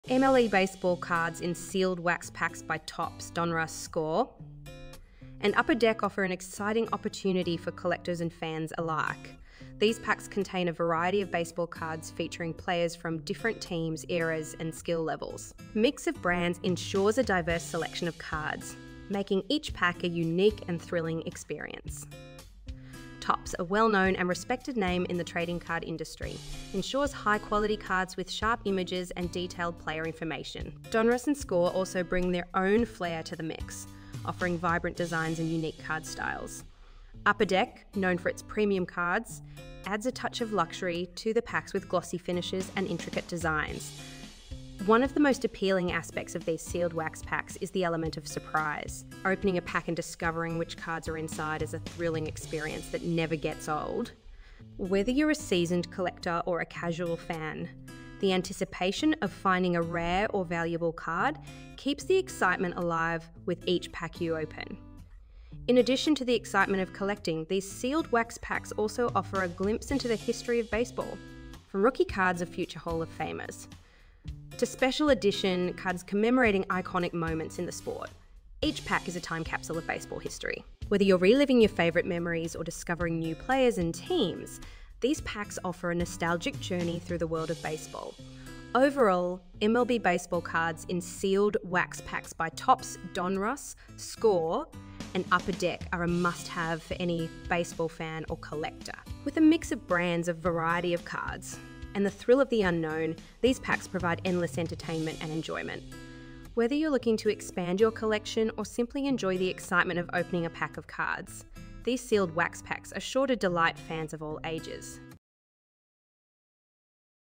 MLB Baseball (100) Cards in Sealed Wax Packs Topps Donruss Score Upper Review
MLB Baseball Cards in Sealed Wax Packs by Topps, Donruss, Score, and Upper Deck offer an exciting opportunity for collectors and fans alike. These packs contain a variety of baseball cards featuring players from different teams, eras, and skill levels. The mix of brands ensures a diverse selection of cards, making each pack a unique and thrilling experience.

Topps, a well-known and respected name in the trading card industry, ensures high-quality cards with sharp images and detailed player information. Donruss and Score also bring their own flair to the mix, offering vibrant designs and unique card styles. Upper Deck, known for its premium cards, adds a touch of luxury to the packs with glossy finishes and intricate designs.

One of the most appealing aspects of these sealed wax packs is the element of surprise. Opening a pack and discovering which cards are inside is a thrilling experience that never gets old. Whether you're a seasoned collector or a casual fan, the anticipation of finding a rare or valuable card keeps the excitement alive with each pack you open.

In addition to the excitement of collecting, these sealed wax packs also offer a glimpse into the history of baseball. From rookie cards of future Hall of Famers to special edition cards commemorating iconic moments in the sport, each pack is a time capsule of baseball history. Whether you're reliving your favorite memories or discovering new players and teams, these packs offer a nostalgic journey through the world of baseball.

Overall, MLB Baseball Cards in Sealed Wax Packs by Topps, Donruss, Score, and Upper Deck are a must-have for any baseball fan or collector. With a mix of brands, a variety of cards, and the thrill of the unknown, these packs provide endless entertainment and enjoyment. Whether you're looking to expand your collection or simply enjoy the excitement of opening a pack of cards, these sealed wax packs are sure to delight fans of all ages.